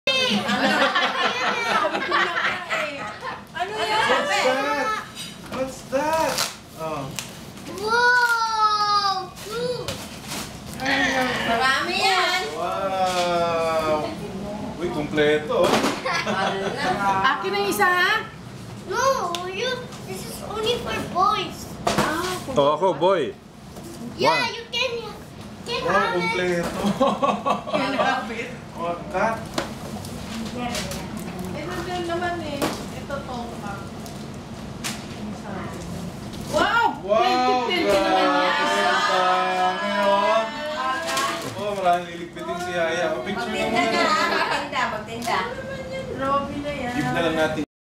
Robby, the youngest Golez grandchild, Xmas gifts opening, 24 December 2010
This is Robby Golez Duque, the youngest grandchild of Roilo & Natty Golez. Being the youngest, he is of course then center of the family's attention as shown by the number of gifts showered on him by the Lola and Lolo and the rest of the family.